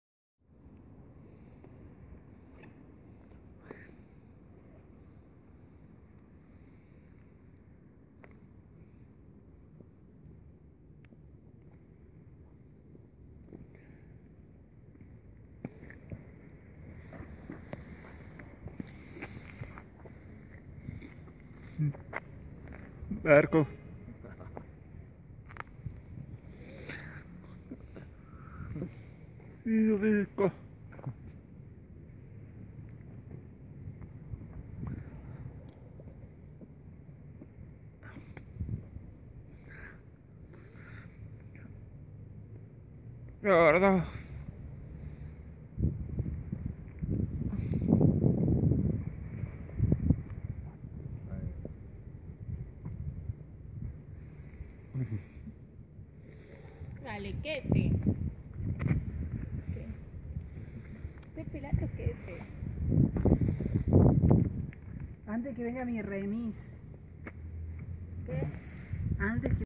"En el centro civico 2015"
La película Sordo llegó en Bariloche
Con mis compañeros y el director.